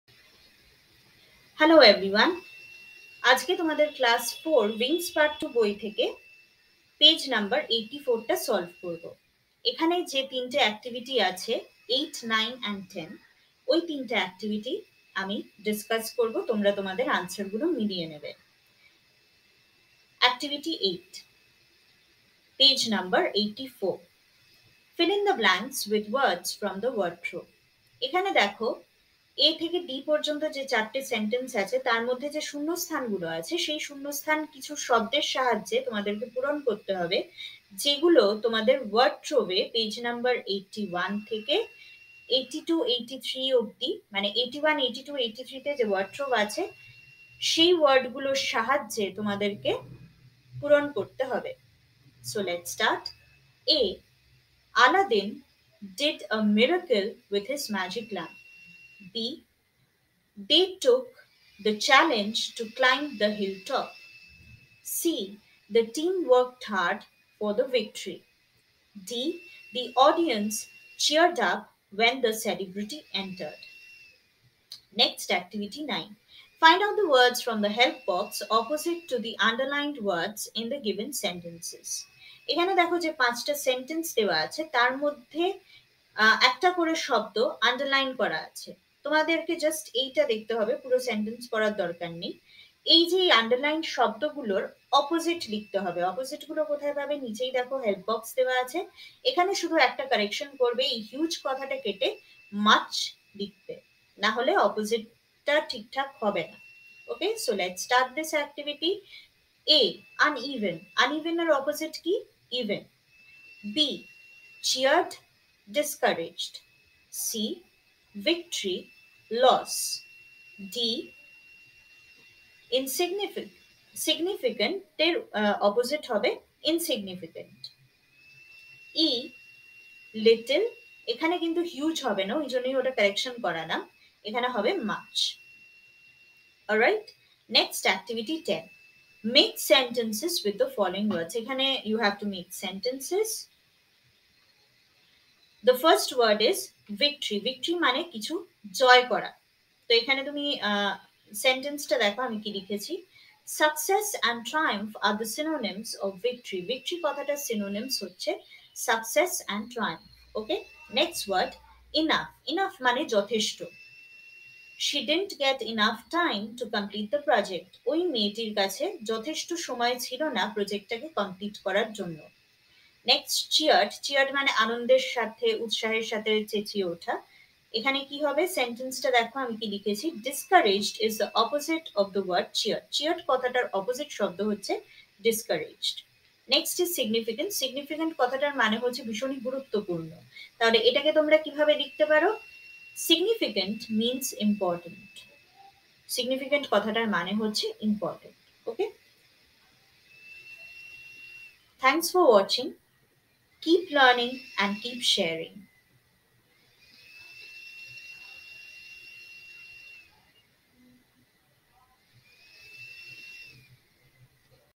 Class 4 // Wings Part - 2 // Page 84 // Activity 8,9,10 // Fully Solved // with correct Answers
Class - IV
English Grammar
Wings Book, Part - II
Page - 84

Activity - 8
Activity - 9
Activity - 10

Explained in Bengali
Fully Solved with Correct answers
Check Your Answers

Primary Education
WBBPE

Learn with Priyanka Chatterjee

English Today

Online English